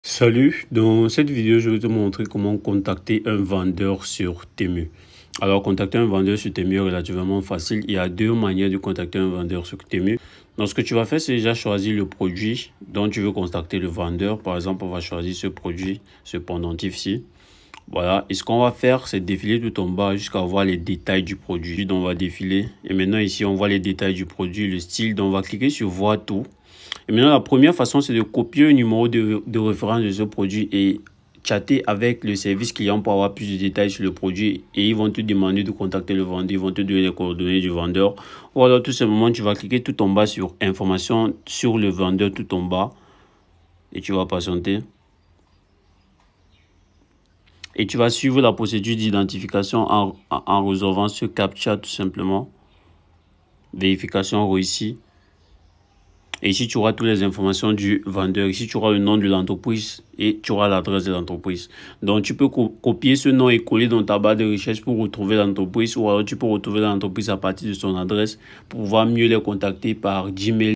Comment Contacter Un Vendeur Sur Temu ( FACILE )
Dans cette video je te montre exactement Comment Contacter Un Vendeur Sur Temu ( FACILE )